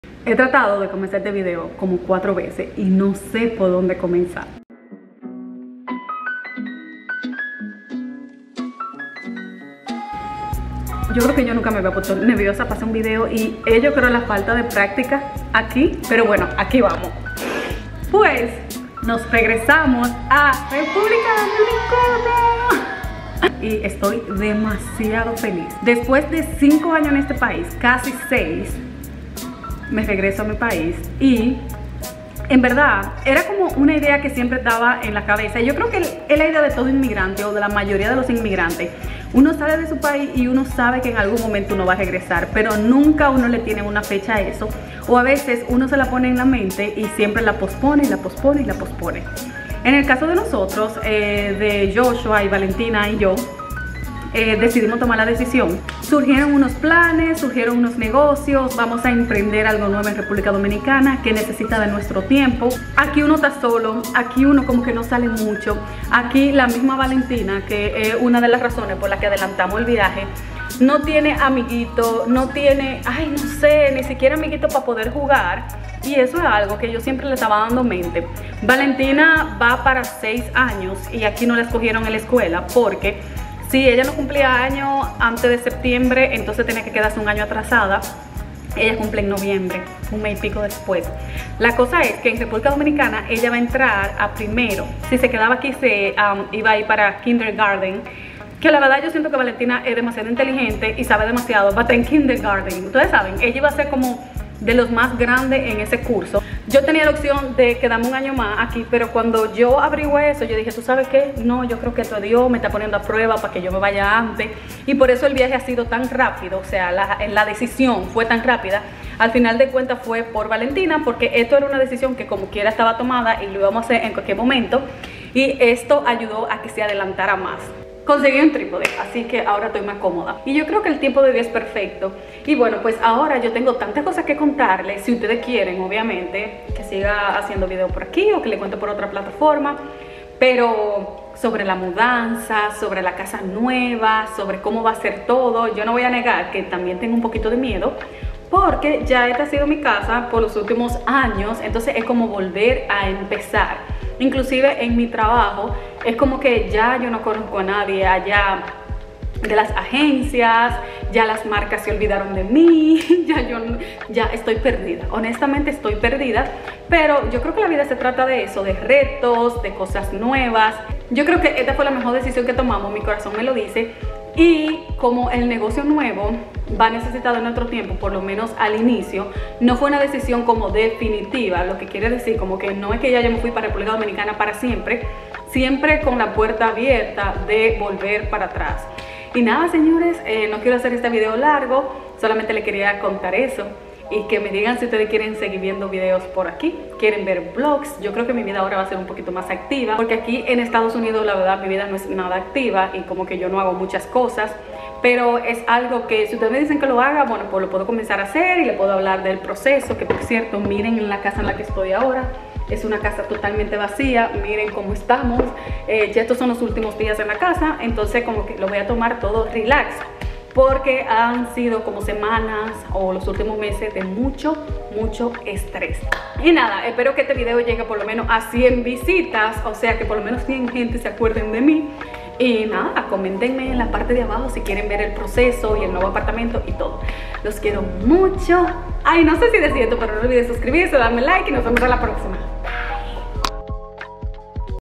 Me mudo a REPÚBLICA DOMINICANA 🇩🇴
después de mucho tiempo sin publicar por qui me emociona volver y contarles que im back to DR